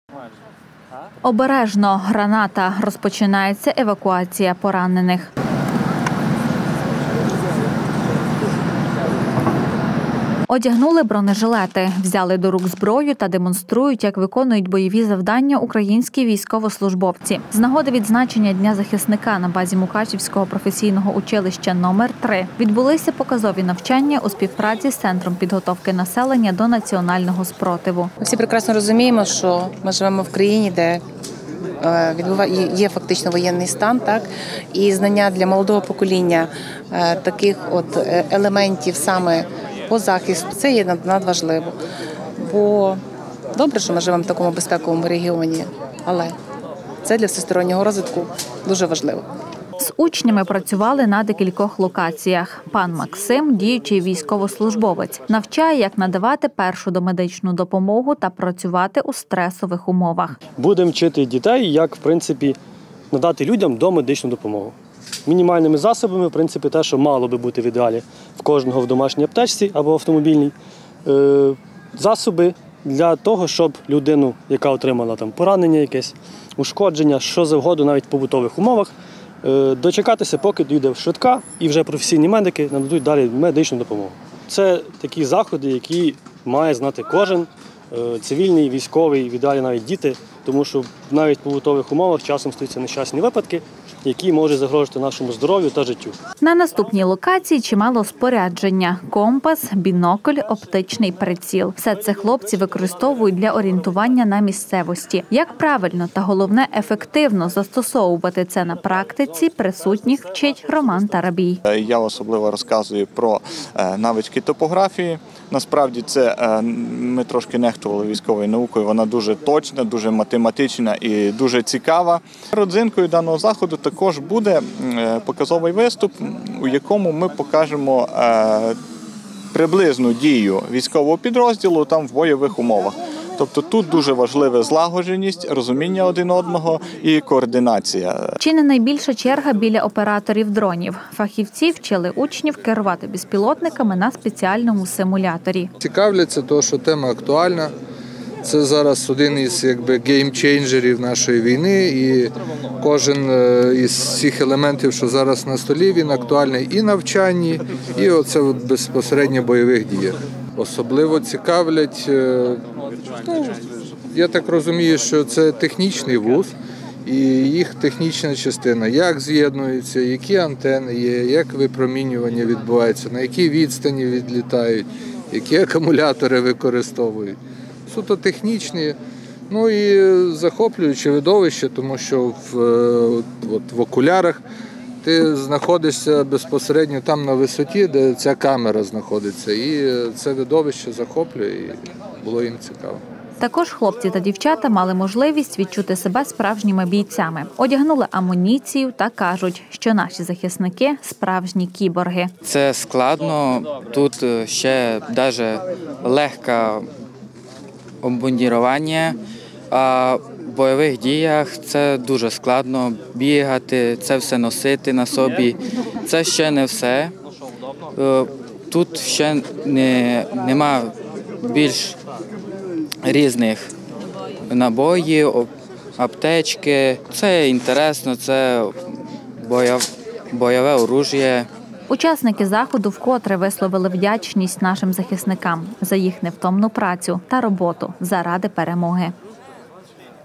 Показові навчання нацспротиву
Обережно! Граната. Розпочинається евакуація поранених. Одягнули бронежилети, взяли до рук зброю та демонструють як виконують бойові завдання українські військовослужбовці.  З нагоди відзначення Дня захисника на базі Мукачівського професійного училища № 3 відбулися показові навчання у співпраці з Центром підготовки населення до національного спротиву. З учнями працювали на декількох локаціях. Пан Максим – діючий військовослужбовець навчає як надавати першу домедичну допомогу та працювати у стресових умовах. На наступній локації чимало спорядження – компас, бінокль, оптичний приціл. Все це бійці використовують задля орієнтування на місцевості. Як правильно та головне ефективно застосовувати це на практиці присутніх вчить Роман Тарабій. Чи не найбільша черга біля операторів дронів. Фахівці вчили учнів керувати безпілотниками на спеціальному симуляторі. Також хлопці та дівчата мали можливість відчути себе справжніми бійцями. Одягнули амуніцію та кажуть, що наші захисники – справжні кіборги. Учасники заходу вкотре висловили вдячність нашим захисникам за їх невтомну працю та роботу заради перемоги.